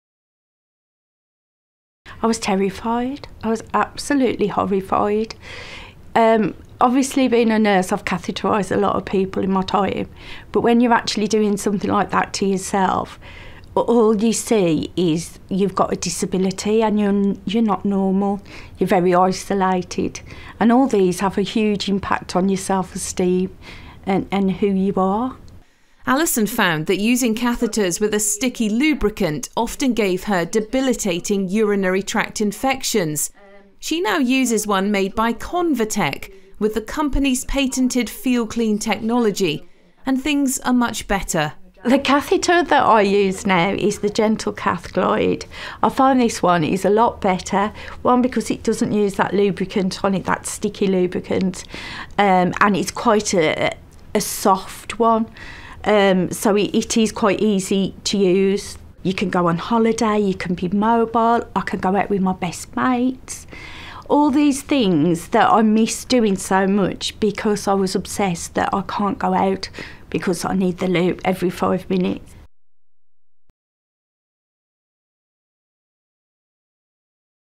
Not all intermittent catheters are the same
Alison found that using catheters with a sticky lubricant often gave her debilitating urinary tract infections. She now uses one made by ConvaTec, with the company’s patented “FeelClean Technology,” and things are much better.